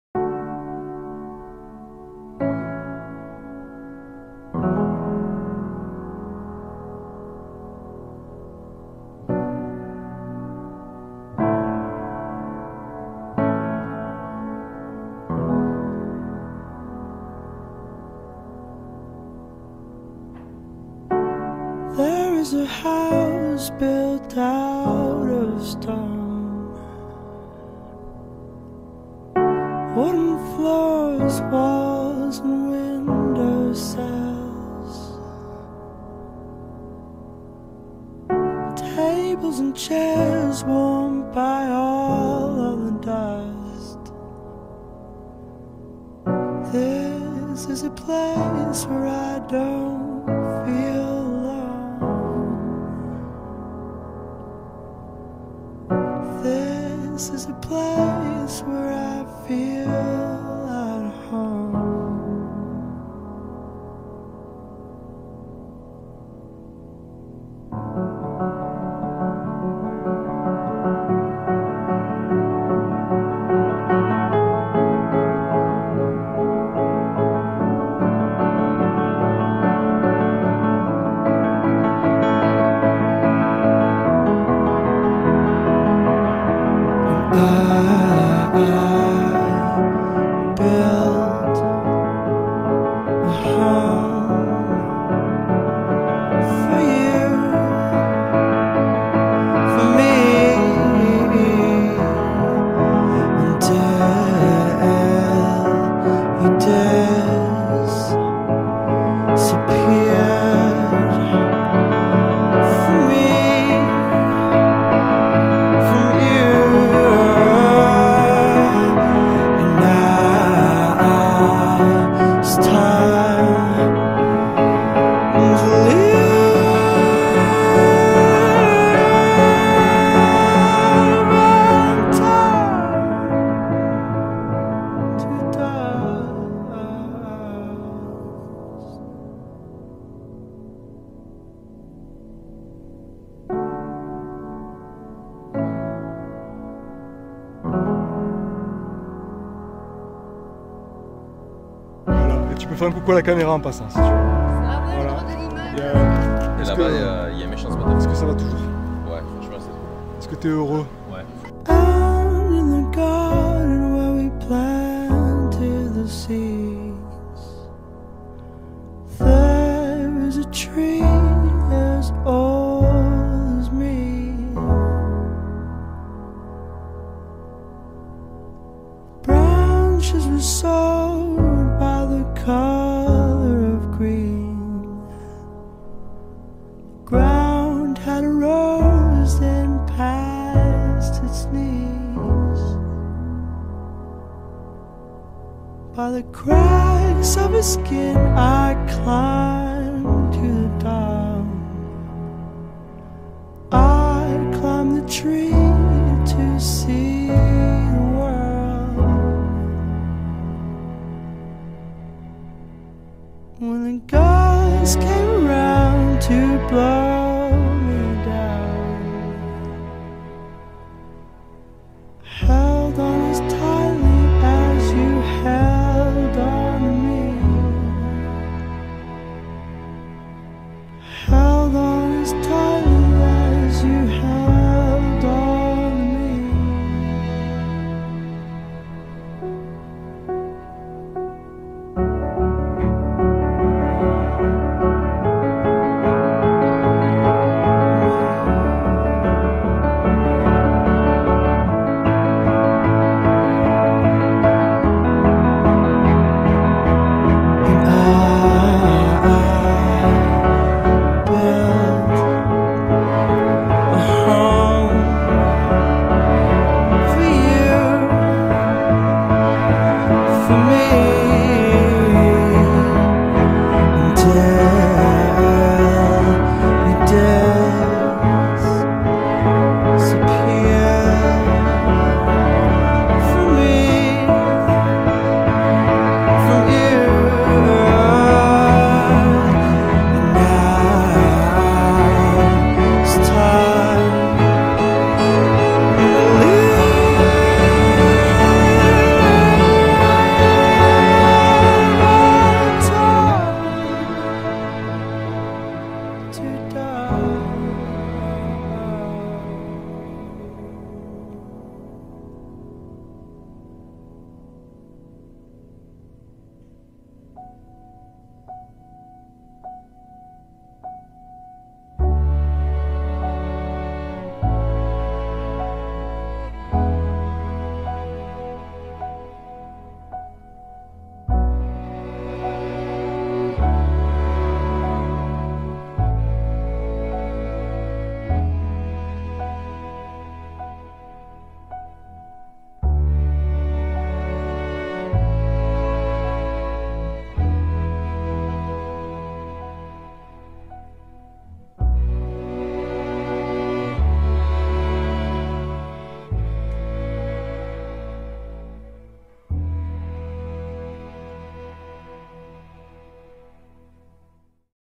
Bienvenue dans le plus grand canyon d'Europe
Vous cherchez un lieu unique avec de l'eau turquoise pour vous ressourcer un week-end ? Alors le sentier blanc Martel est le bon endroit !